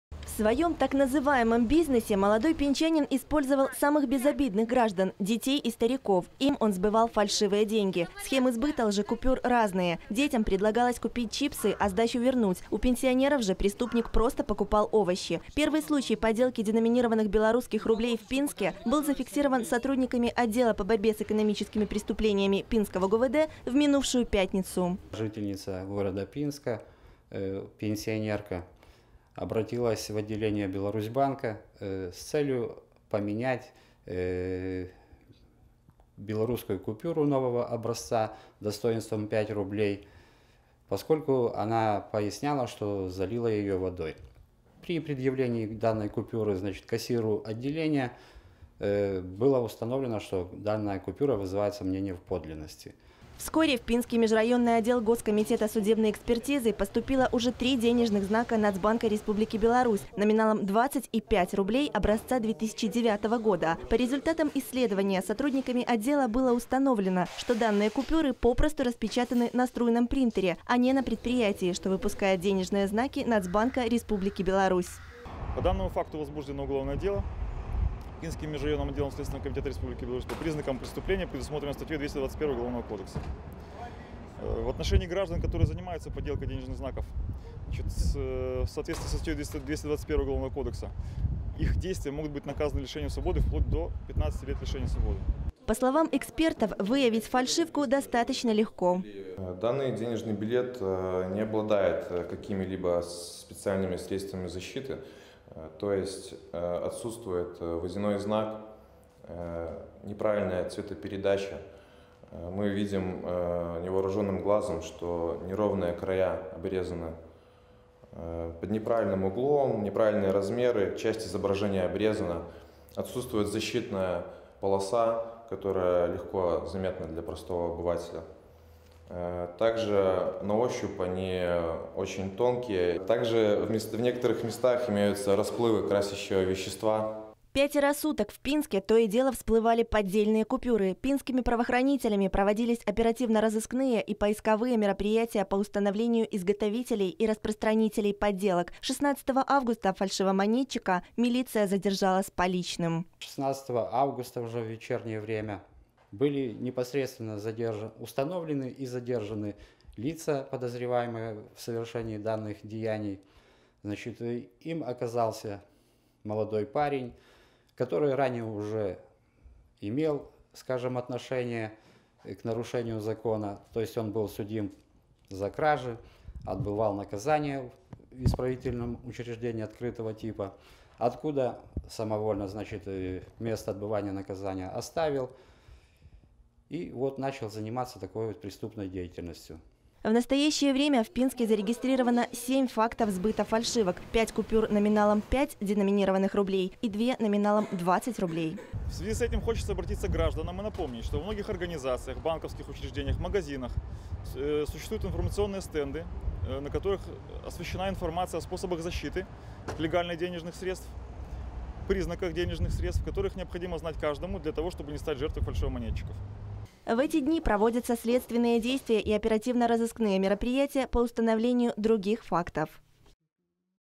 Случаи подделки новых денежных купюр выявлены в Пинске
«Не имей сто рублей, а имей миллионы. Любым способом». Наверное, так думал двадцатилетний фальшивомонетчик, которого на днях задержали в Пинске. Молодой человек печатал поддельные купюры на струйном принтере, после чего хитрыми способами их сбывал.